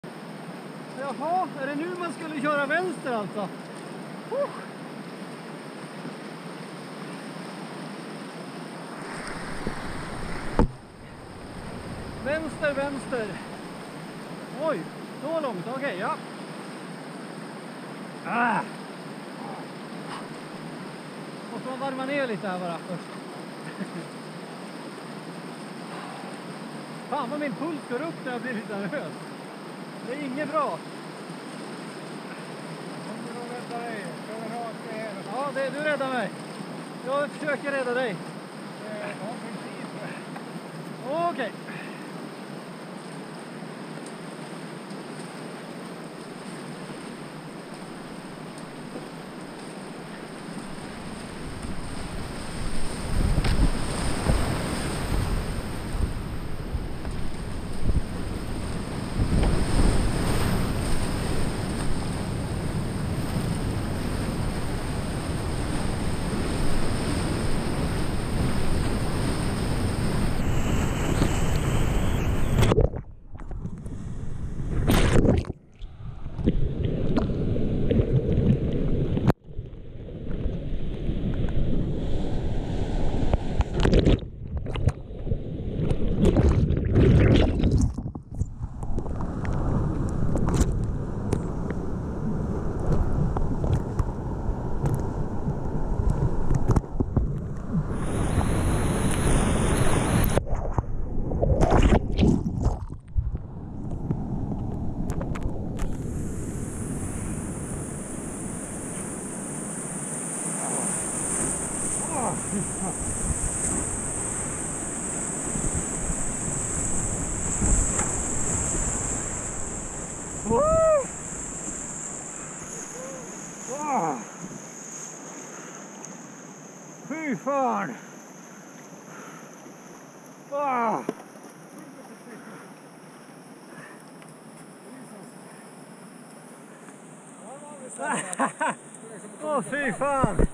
"Vänster-vänster"-linjen i Vävaren, Testeboån, 2021-04-07
Jag och Tomas blev lite djärva och bestämde oss för att prova en ny linje i Vävaren vilket innebar att hålla höger i droppet före "Pelardroppet" och sedan även höger i själva "Pelardroppet" och där försöka boofa över hålet nedanför. Normalt så kör man båda droppen till vänster så detta var en helt ny och oprövad linje för oss.

Med facit i hand så kan jag inte rekommendera att köra denna linje om man har svårt att skilja höger från vänster och dessutom är dålig på att boofa...

Varning för svordomar. :-)